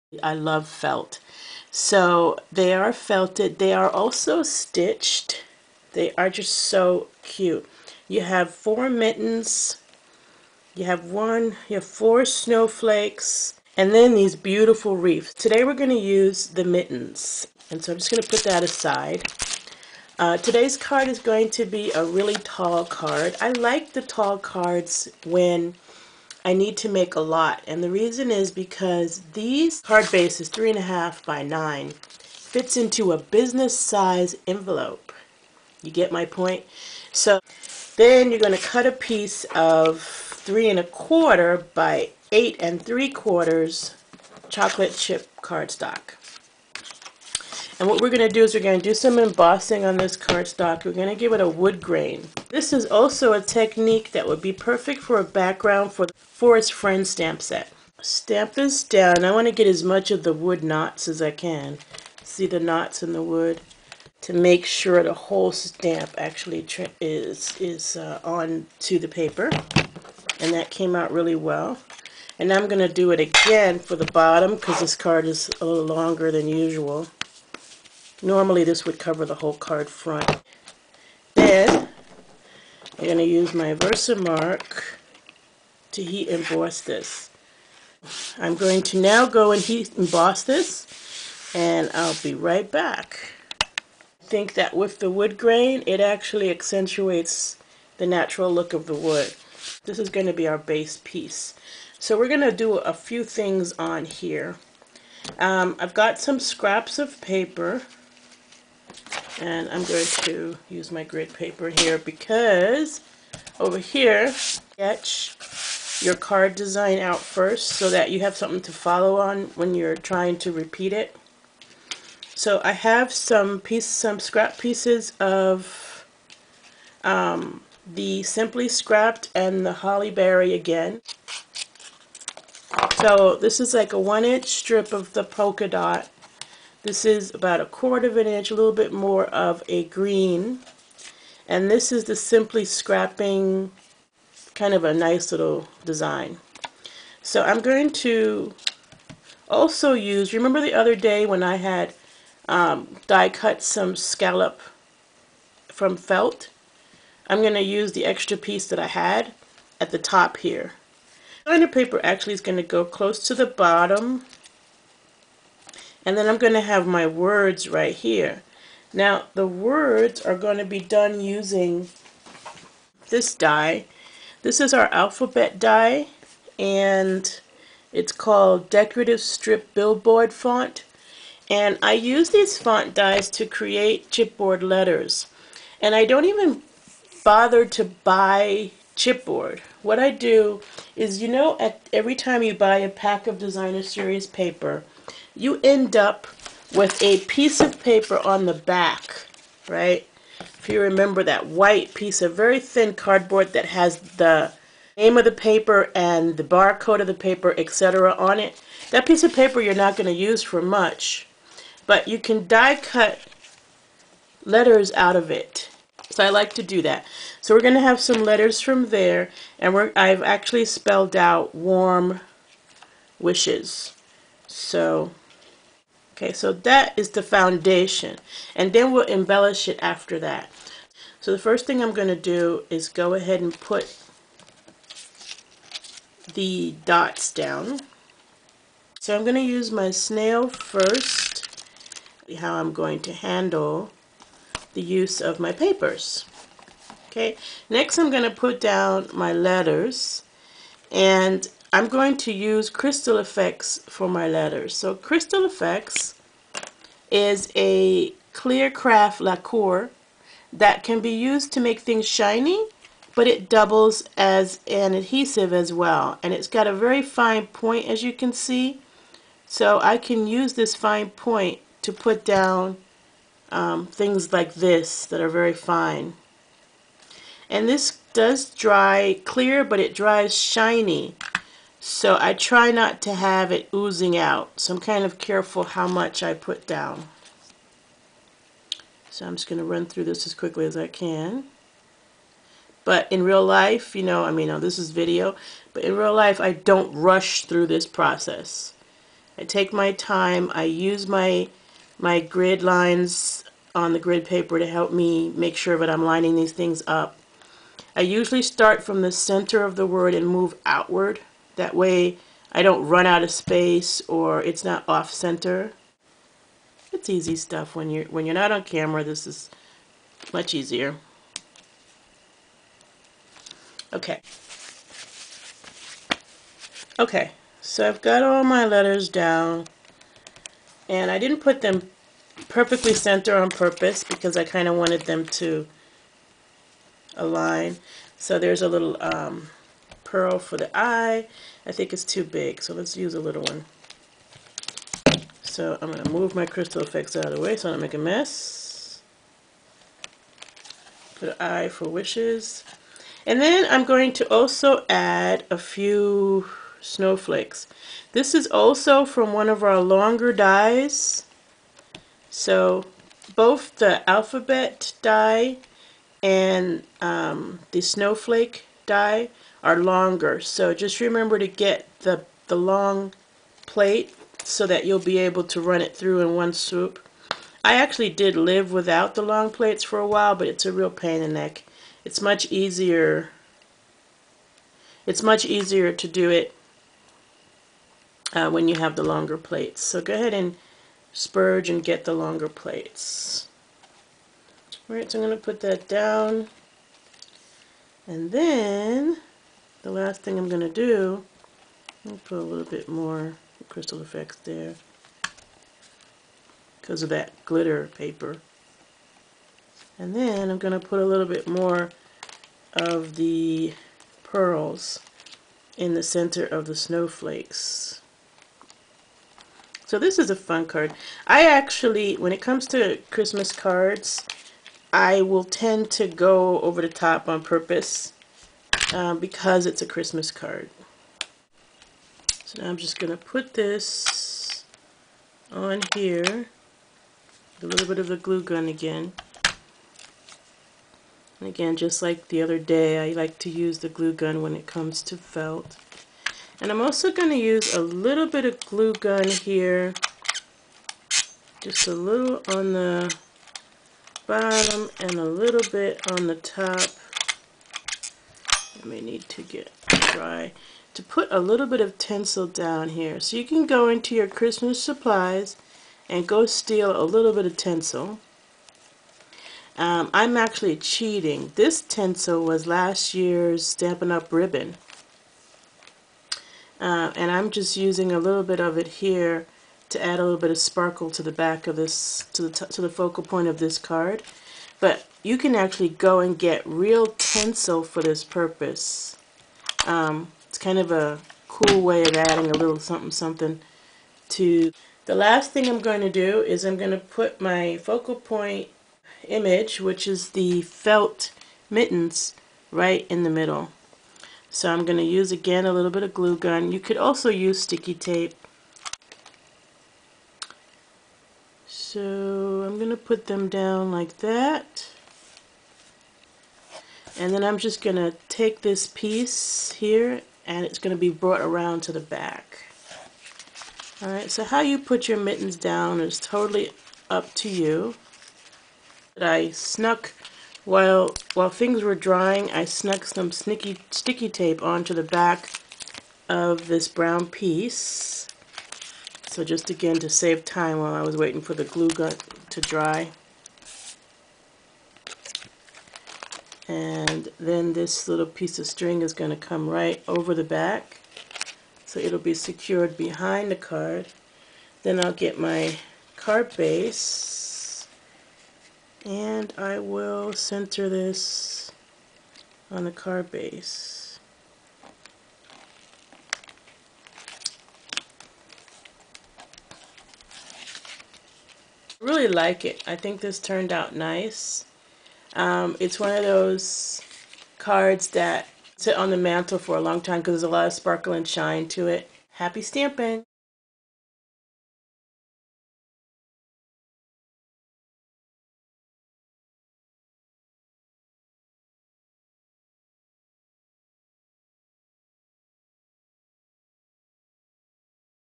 Warm Wishes Felt Accessories
As another response to Sundays haul video, today I explore a very tall Christmas card design (3 1/2 x 9").  With this one feel free to repurpose bill envelopes (green idea!)- or pick up a box of inexpensive business size envelopes for mailing.  I will be posting two more haul responses; one on Friday and another on Saturday.

Holiday Mini Supplies Used:
Stitched Felt Winter Wishes: page 30 #124108
Holly Berry Bouquet DSP: page 20 #124004
Simply Scrappin' Set: page 26 #124007

Catalog Dies found on page 225 of the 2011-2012 Idea Book and Catalog:
#114510 Billboard Font
#120901 Northern Frost
(for info on the longer Big Shot plates see page 215